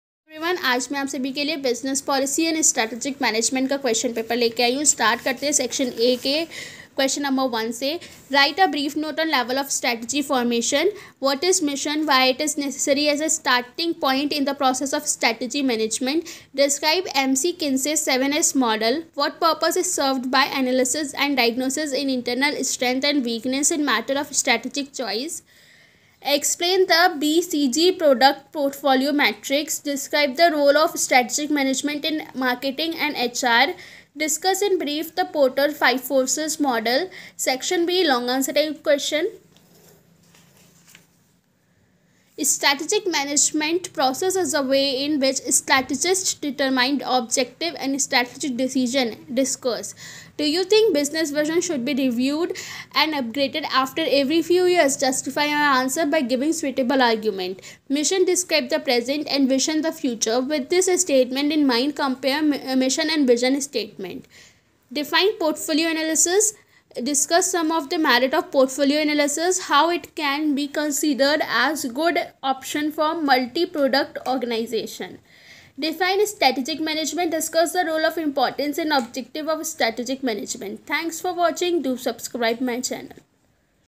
Business Policy and Strategic Management रणनीतिक योजना एक विश्लेषणात्मक गतिविधि question paper
This video is regarded  to the question paper of B.COM (HONOURS) BUSINESS POLICY AND STRATEGIC MANAGEMENT ".